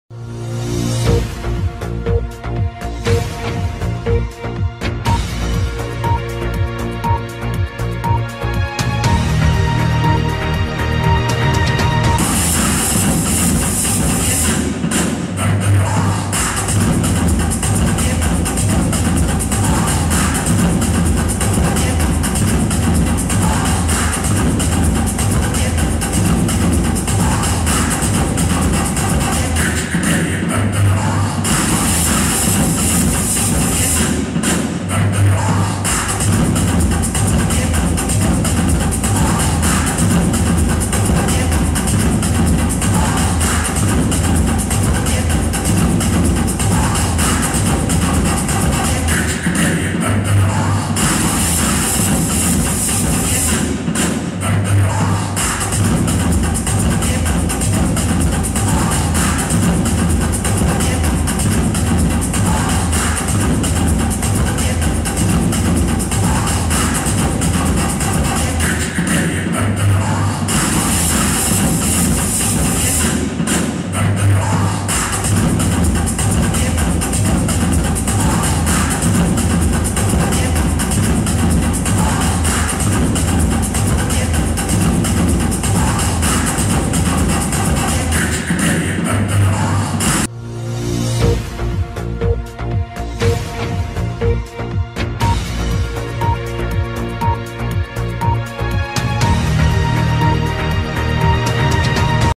VIDEO NOTA LOBOS BUAP 19 DE JULIO
Presentación del uniforme de los lobos BUAP en el complejo cultural universitario.